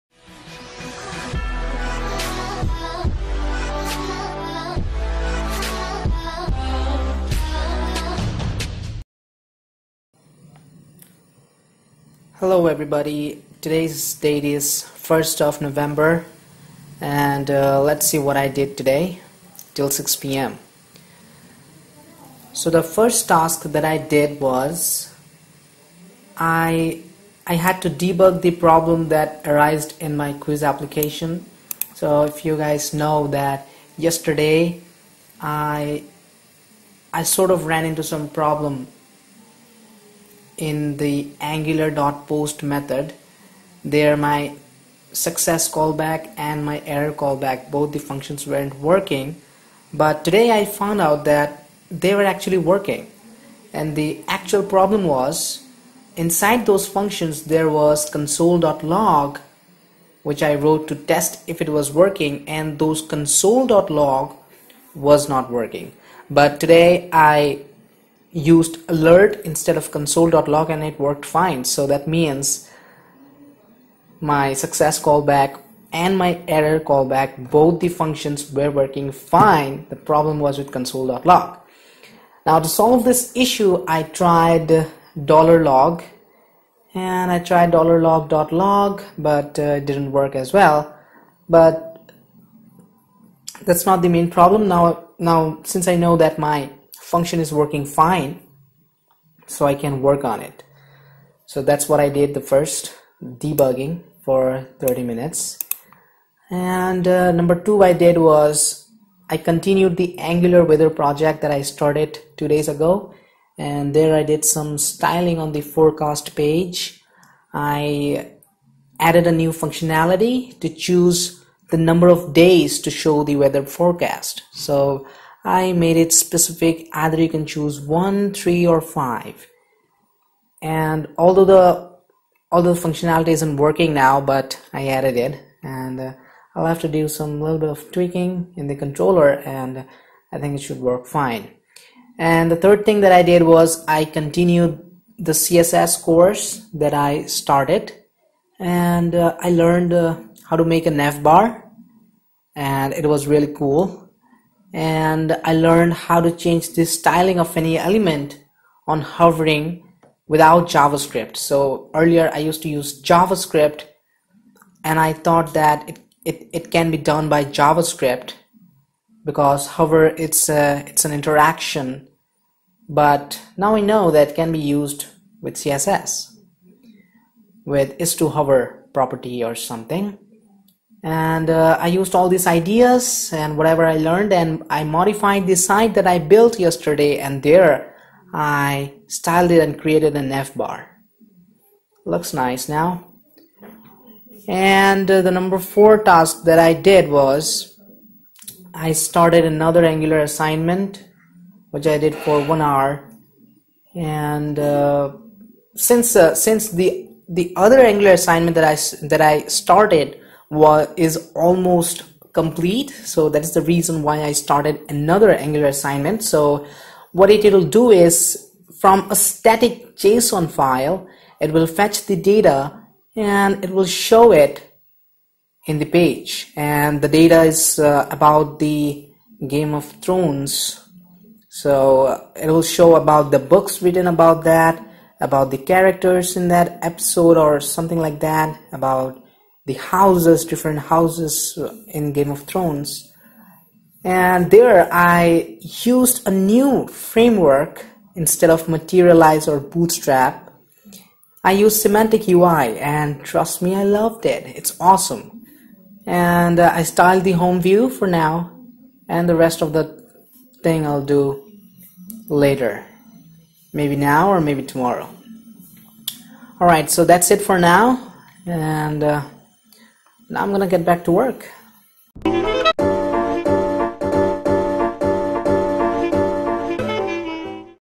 Angular console.log isn't working ! 1st Nov tasks
1st November- Today's Tasks [ TILL 6 p.m ]

1. Debug the problem arised in the quiz application - 30 min

   i)  There I realised $http.post .success callback function and
       the error callback function both were working fine but
       the problem was console.log wasn't printing anything
       in the console

   ii) Used $log still can't print stuff in the console

2. Continue the Angular Weather project - 30 min

 i)   Did some style on the forecast page

 ii)  Added a new functionality to choose number of days
      to show weather forecast.

3. Continue the CSS course - 1 hour

 i)  Learned how to make a nav bar

   ii)  Learned how to change the styling of any element
     on hover without javascript using :hover

   iii) Used all these ideas to modify the site and styled it

4. Started another Angular assignment - 1 hour

 i)  An SPA which will show data from a static JSON file

 ii) Did the basic setup -(used Semantic UI) LOVED IT !

   iii) Styled the home view